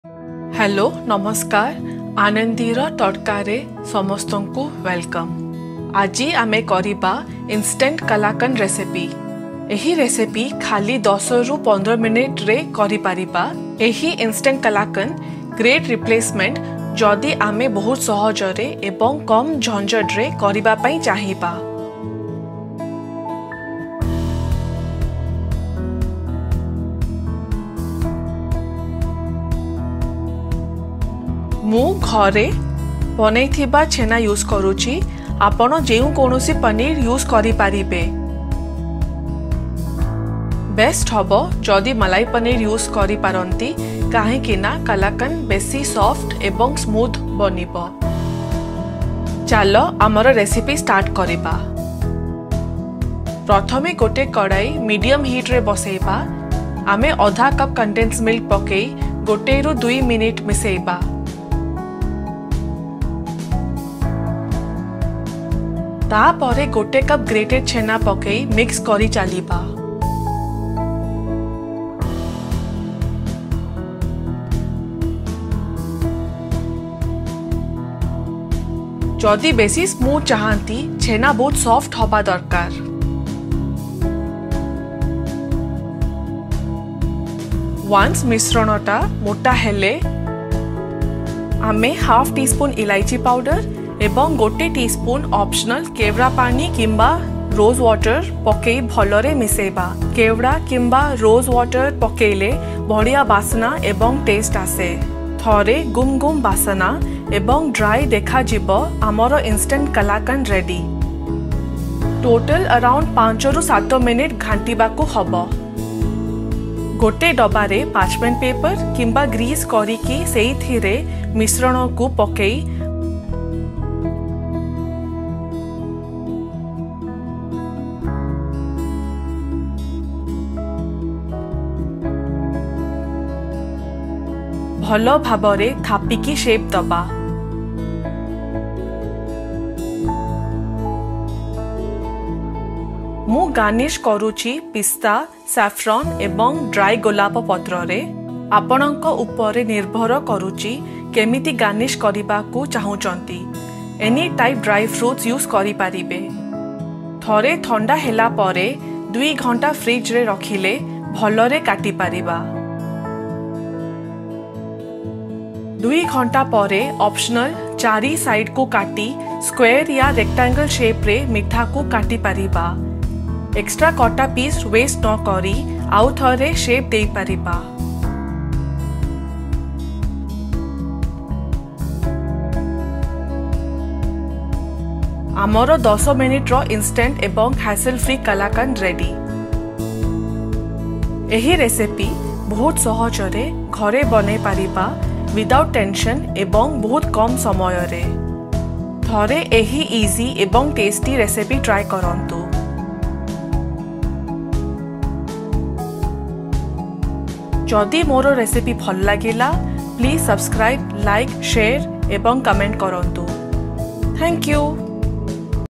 10 ମିନିଟରେ Instant କଲାକନ୍ଦ | INSTANT KALAKAND IN 10 MINS | ODIA
I love Kalakand but the traditionally prepared Kalakand recipe is done by evaporating and solidifying the milk; then it is curdled, to which sugar is added and then cut to shapes. So it is a lengthy and time-taking process. Instead here is a hassle - free recipe for Instant Kalakand using soft and moist paneer and condensed milk. Do try this delicious and quick recipe.

Makes Around 4- 6 Pieces

Ingredients:

½ Cup Condensed Milk
1 Cup Grated Soft Paneer – Preferably Malai Paneer
½ tsp Cardamom
1 tsp Kewra Water / Rose Water (Adding this gives an amazing smell and taste)

Garnishing (Optional):
Pistachio (Can use any dry fruits)
Saffron Strands
Dried Rose Petals

Youtube Icon picture Credit Designed by vectorpocket / Freepik
Youtube Banner & End picture Credit Designed by Freepik